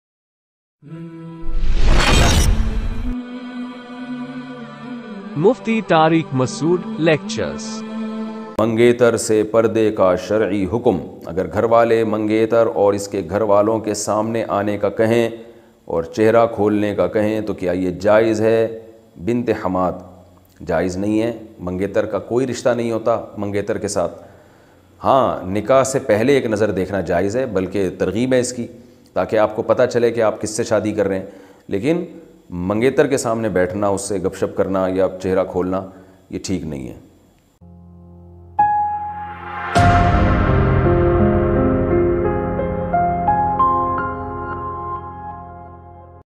Mangetar Se Parde Ka Shari Hukum | Mufti Tariq Masood Lectures
Mangetar Se Parde Ka Shari Hukum |

➤ For Masail Program Join us on Ask Mufti Tariq Masood